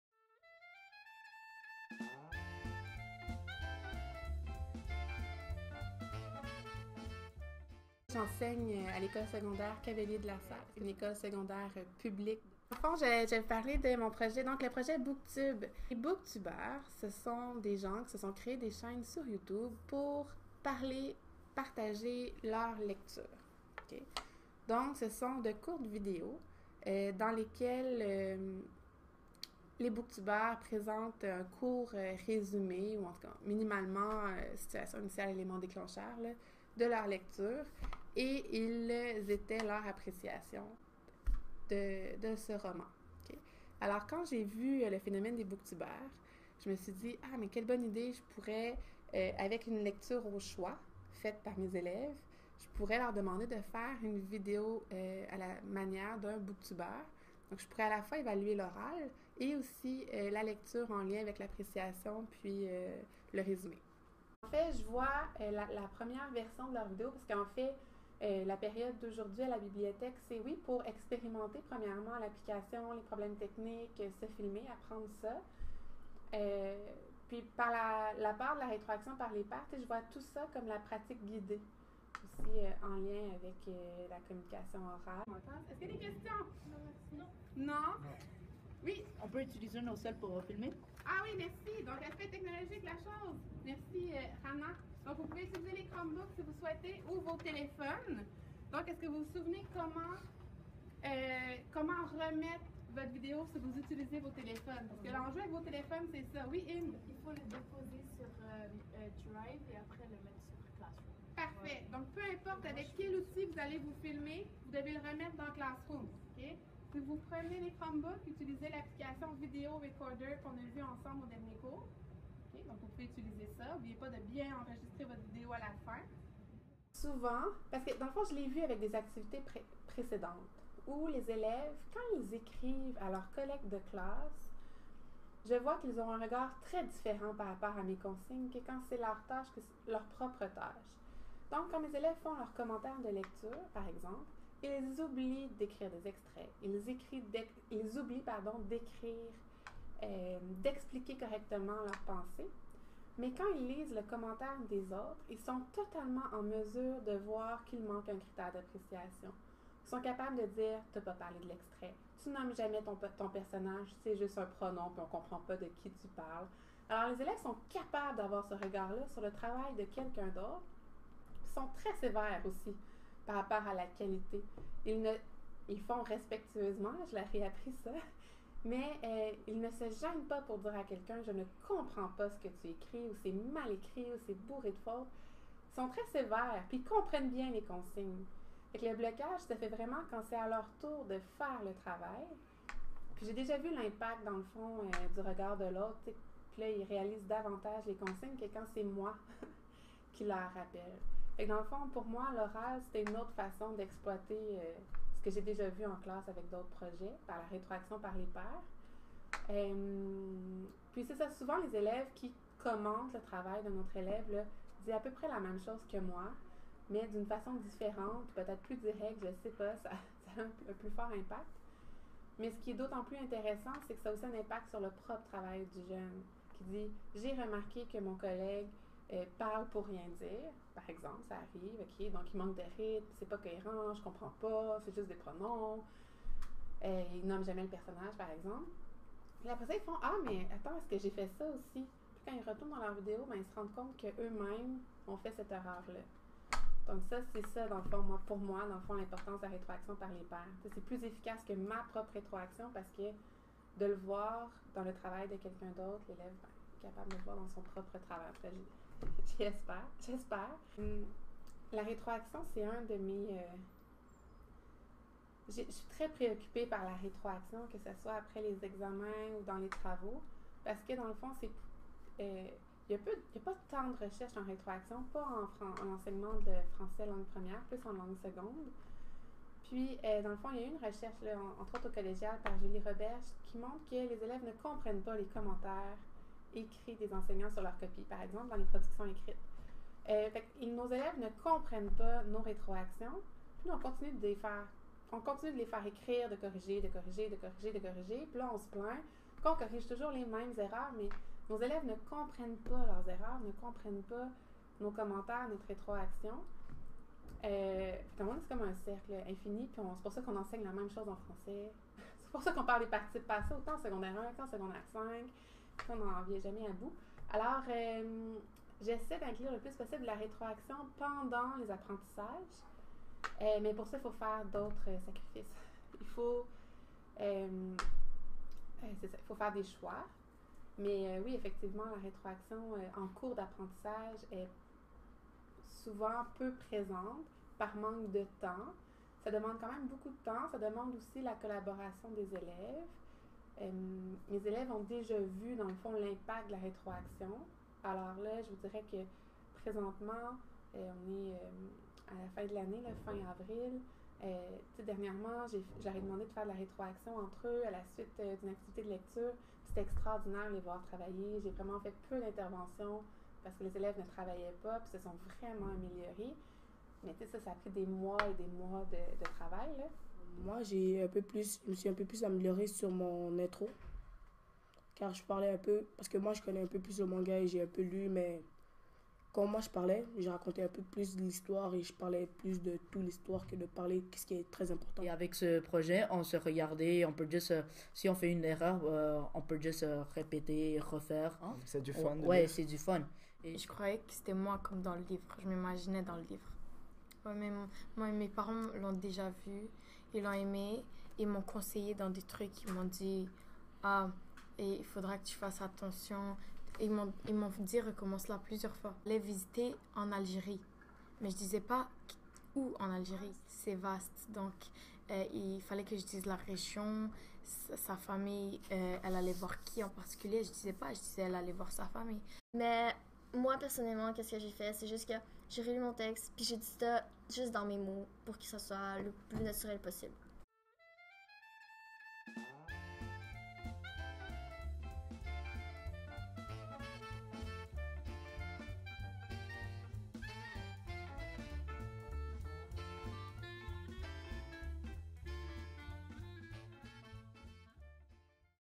Impacts de la rétroaction par les pairs en projet BookTube
Cette vidéo traite de des impacts de la rétroaction par les pairs en projet BookTube.